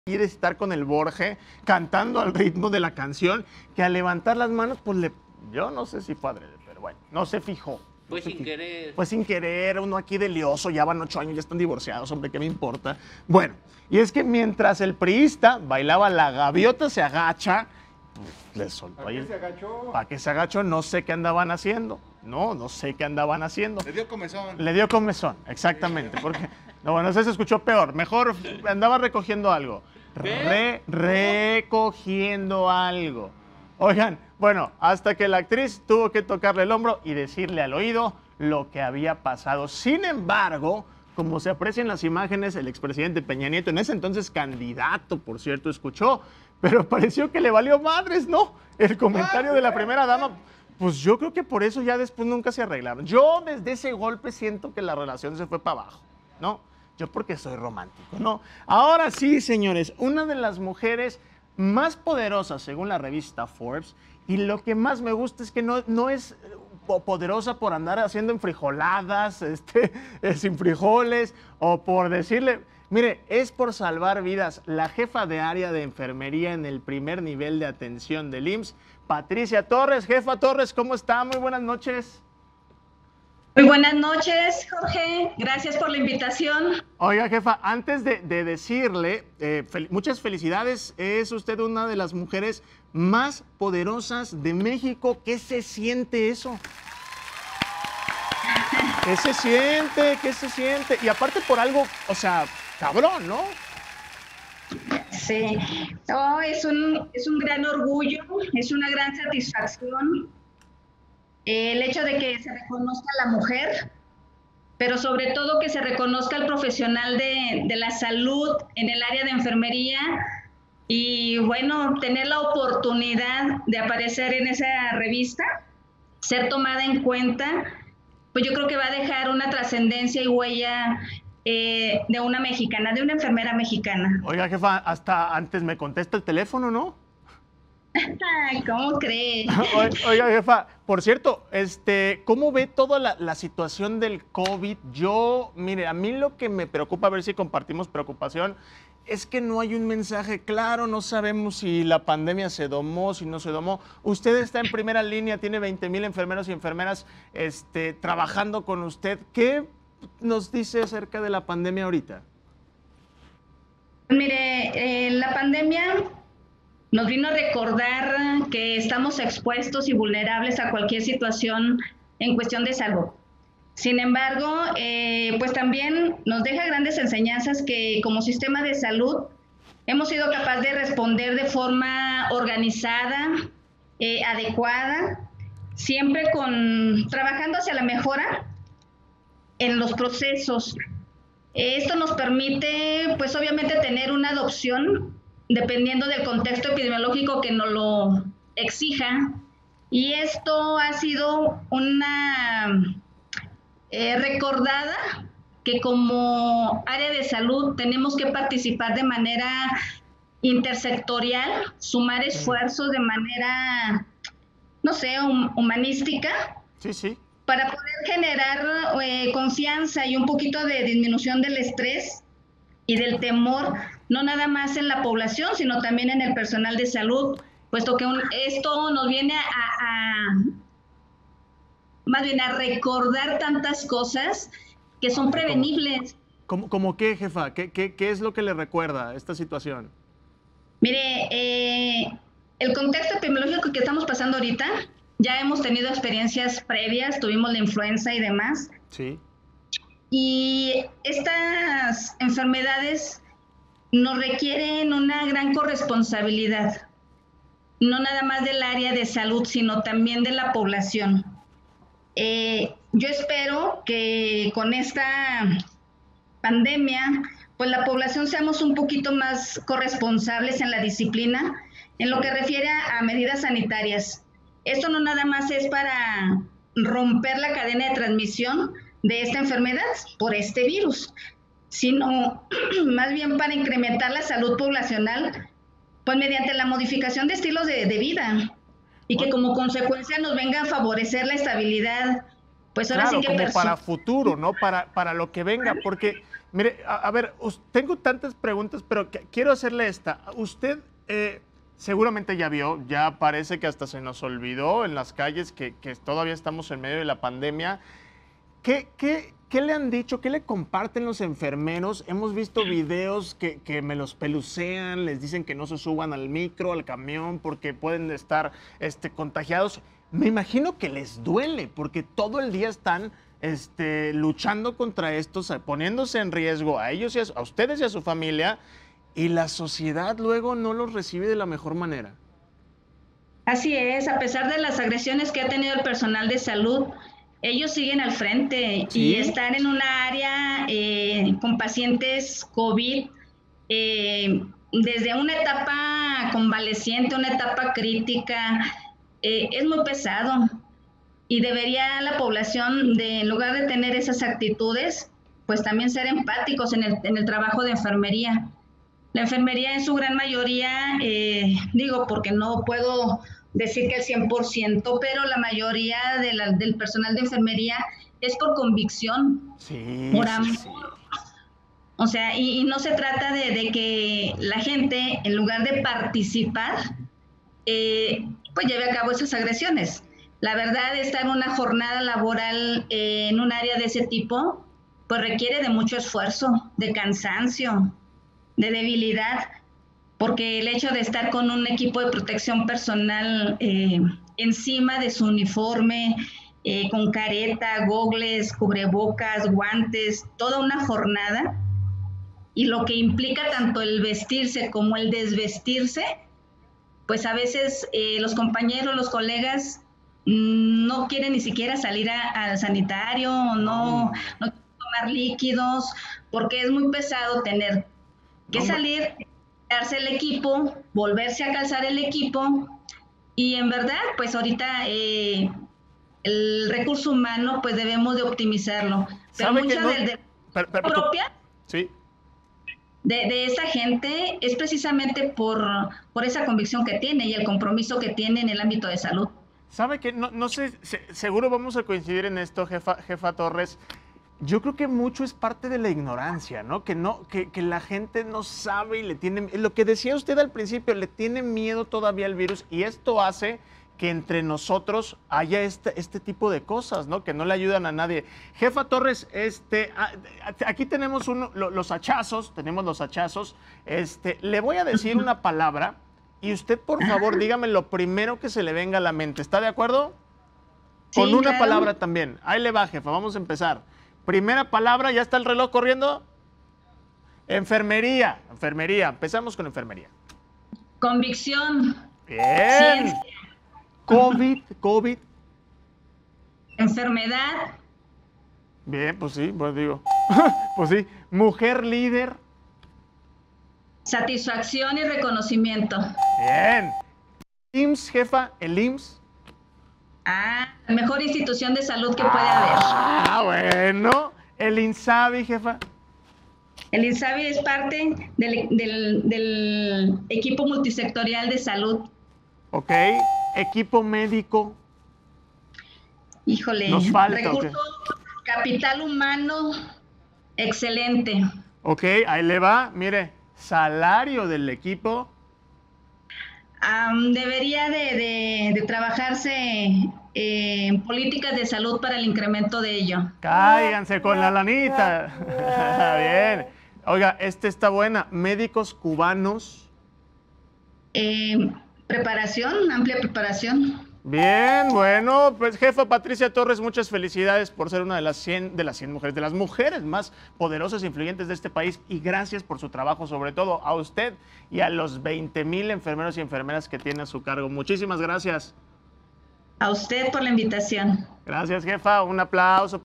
ConHDeHacha | La pandemia nos recordó lo vulnerables que somos: Jefa de enfermeras del
En su edición de junio la Revista Forbes México da a conocer el listado de las 100 mujeres más "Poderosas 2020 en México", una de las personas que conforman la lista, es la jefa de enfermeras del IMSS, la Jefa Patricia Torres, quien cuenta con 29 años de la experiencia profesional, aseguró que es un reconocimiento muy lindo, pero se considera una más de las 131 mil enfermeras adscritas al seguro social.